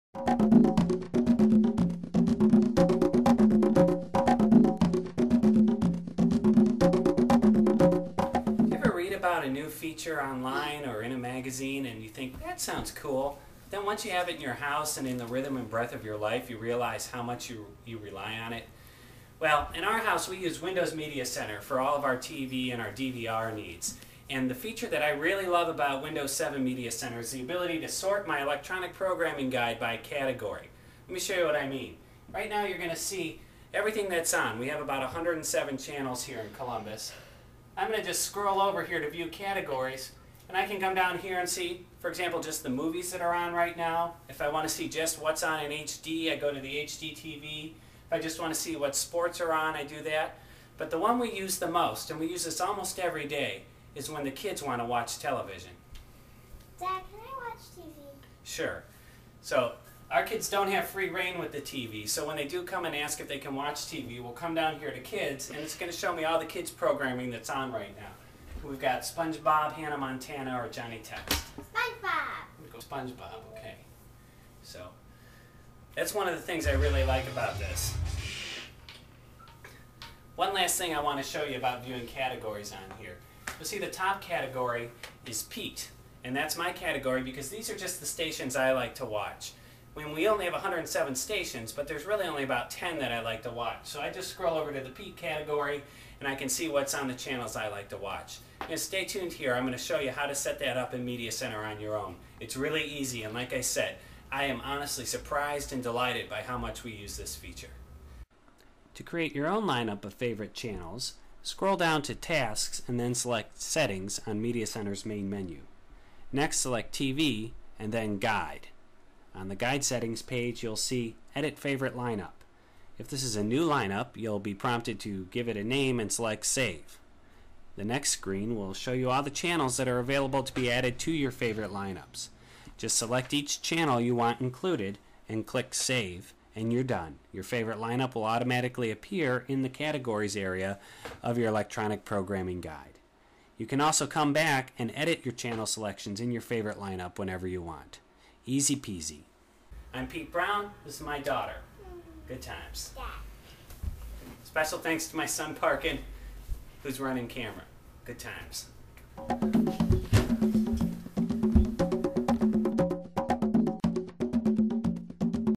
Your channels, your way: Custom channel lineups in Windows 7 Media Center
Overview of categories in the Electronic Programming Guide in Windows 7 Media Center, and how-to set-up a custom channel lineup of your own.